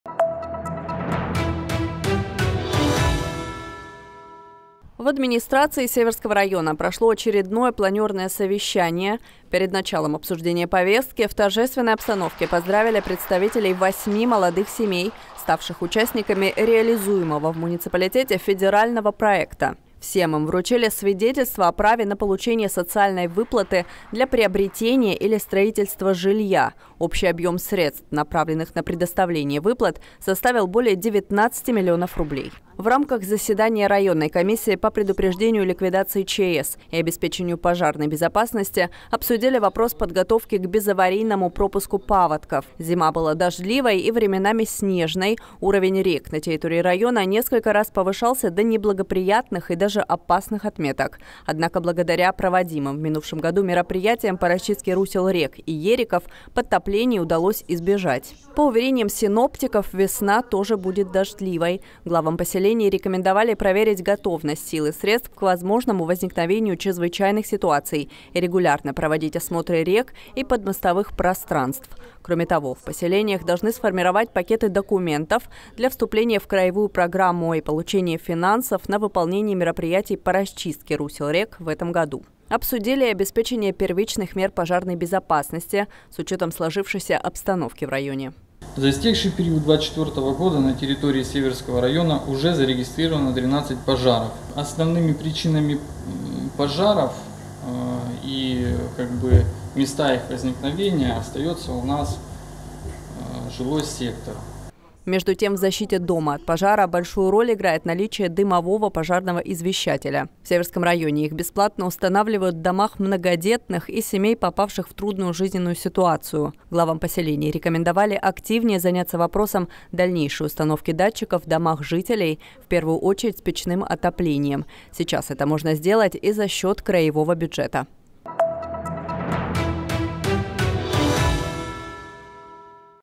Молодые семьи в Северском районе получили социальные выплаты для приобретения жилья
Перед планерным совещанием, прошедшим в администрации муниципалитета, в торжественной обстановке представителям восьми семей, проживающих в сельской местности, вручили  свидетельства о праве на получение социальной выплаты. Использовать ее можно при покупке, или строительстве дома. Общий объём направленных средств составил более 19 млн рублей. Стать участниками реализуемого в муниципалитете федерального проекта могут семьи, где возраст супругов не превышает 35 лет и имеется подтвержденная нуждаемость в улучшении жилищных условий.